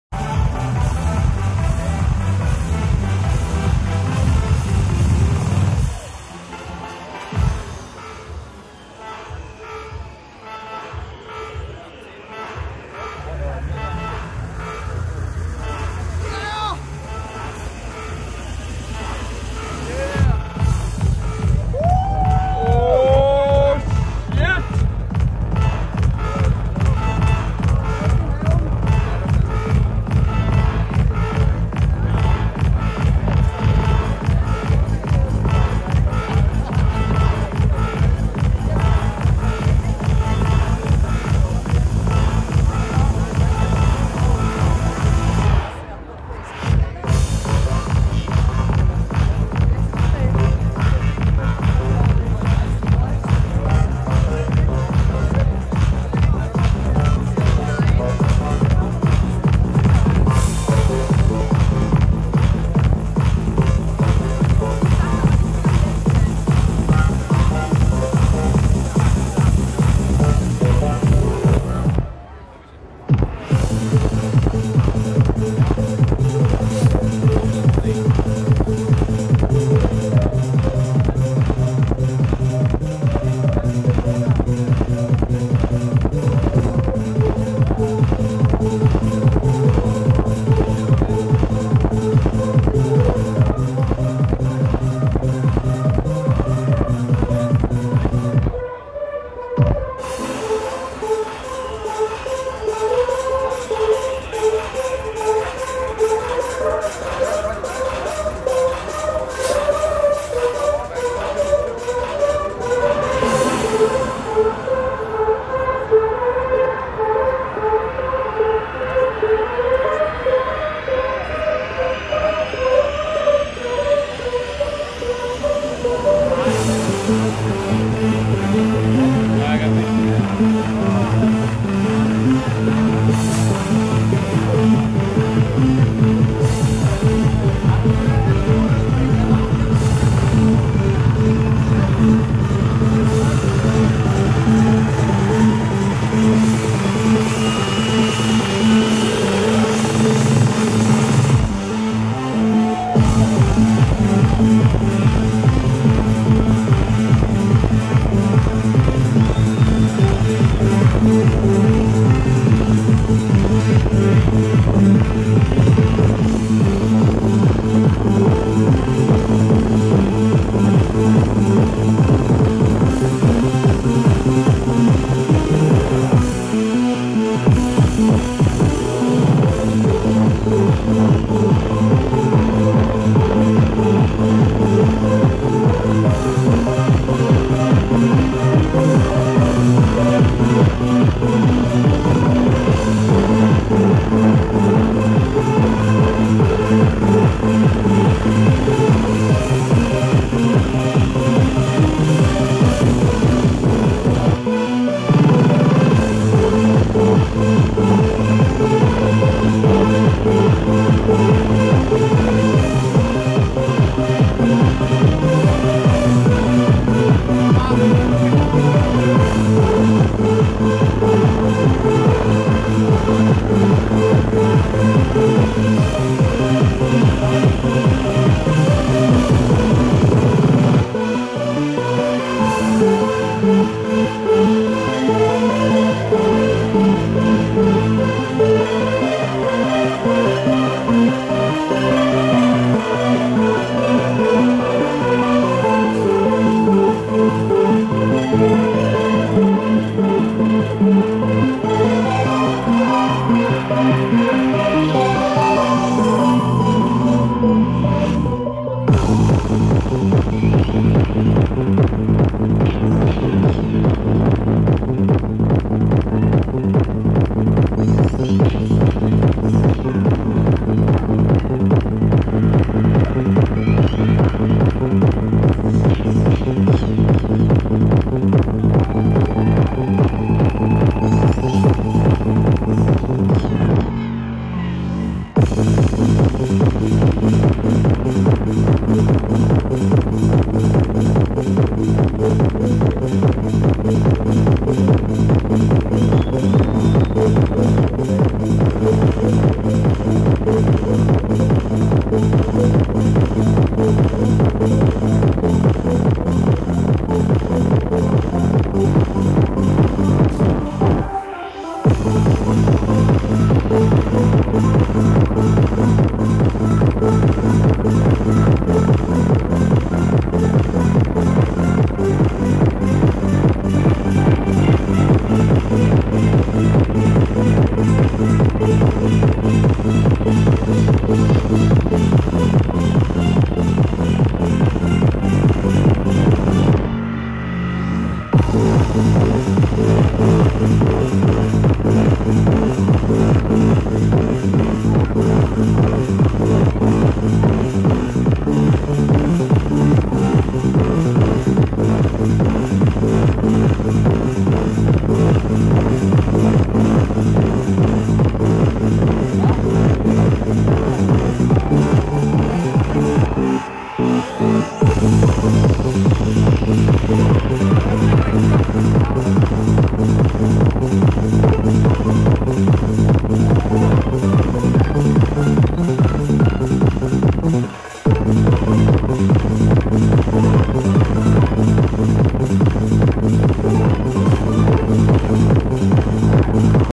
Talamasca LIVE @ Fullmoon Festival 2010, Wittstock - ♪♫♪♫ - High Def
A little video from TALAMASCA's set on Fullmoon Festival (FMF) 2010 in Wittstock, Germany. A big "Thank you" to the organizers behind FMF (it was my 4th time this year) and to all the happy people going there year ater year. You make the trip from Denmark worth it, each and every time :) Check my other trance videos from festivals around Europe